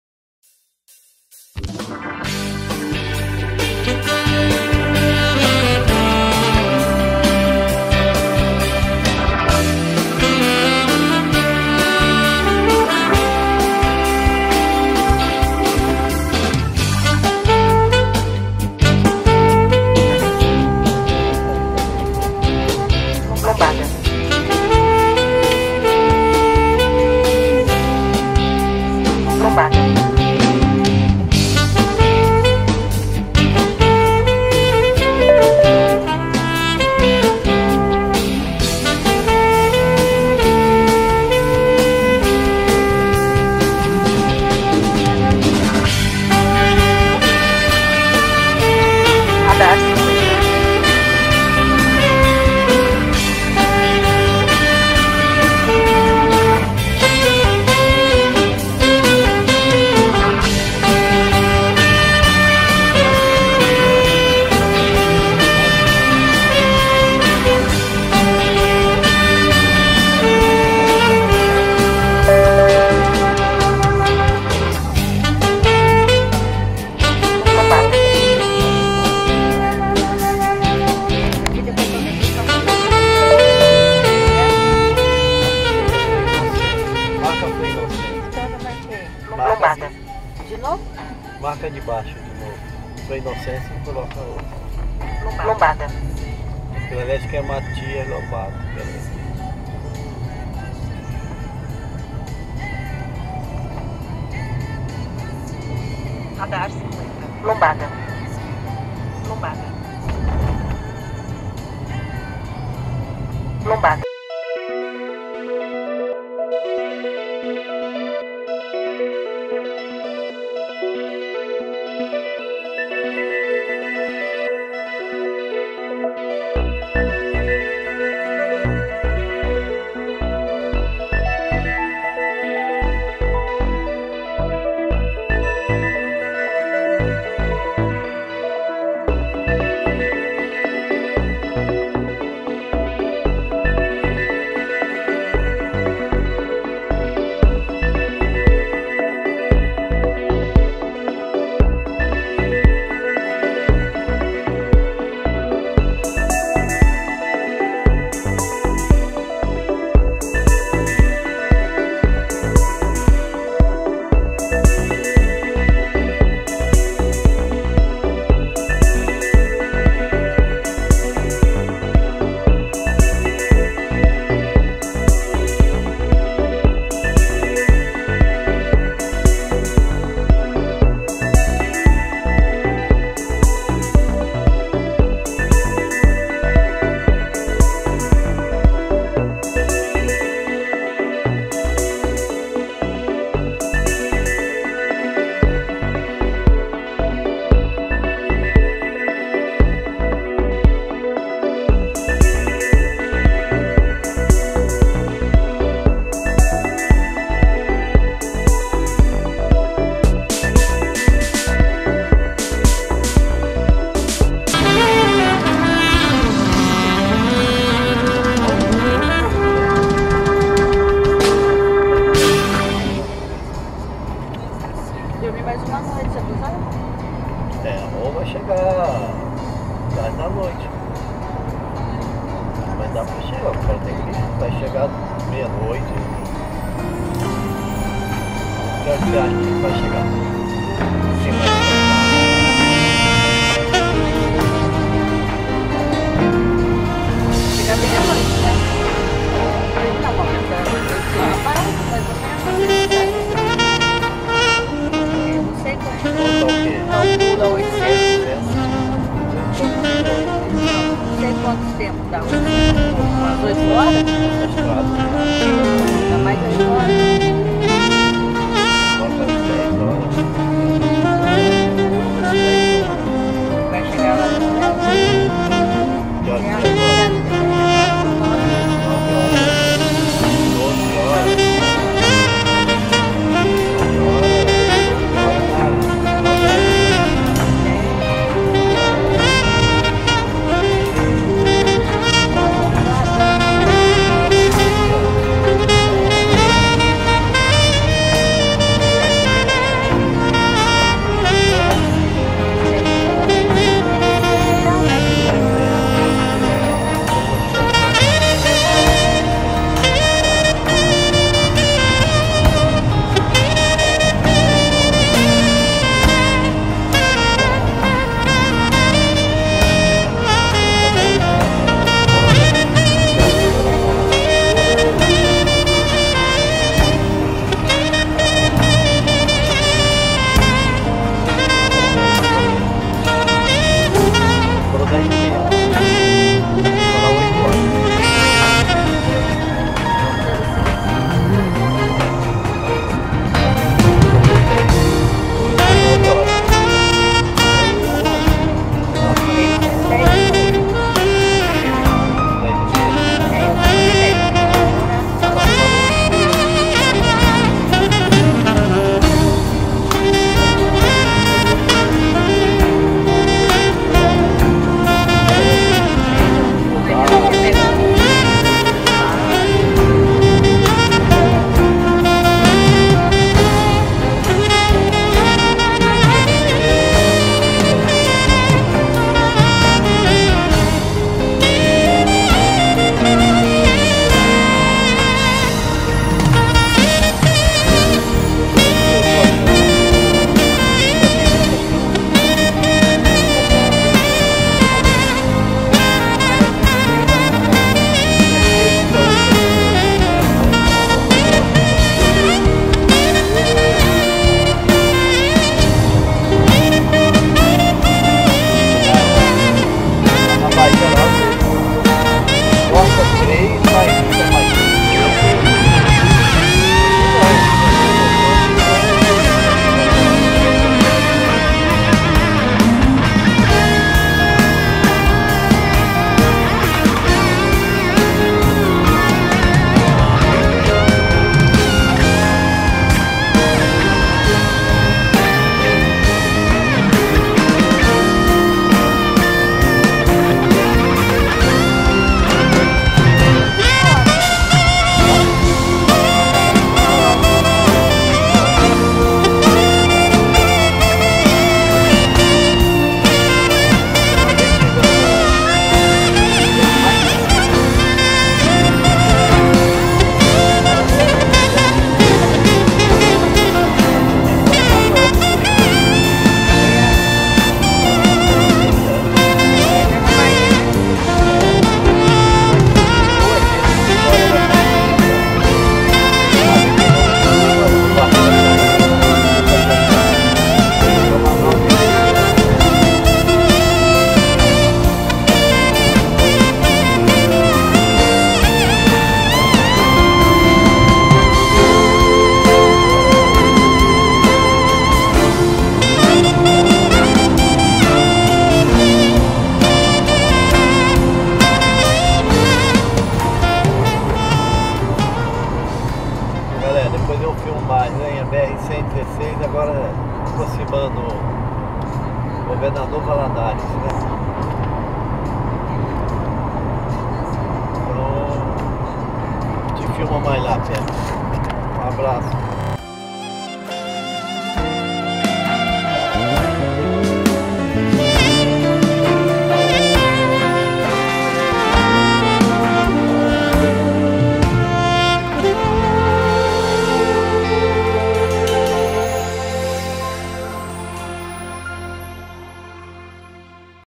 Viagem ao Nordeste 2019 Retorno (60° Parte), Passagem na Cidade de Mathias Lobato(MG) Rodovia BR 116
Viagem ao Nordeste 2019, Passagem na Cidade de Mathias Lobato ( MG ) e Região, pela Rodovia Santos Dumont ( BR 116 ) sentido ao Sul ( Caratinga / MG ). Continuação: ( 61° Parte )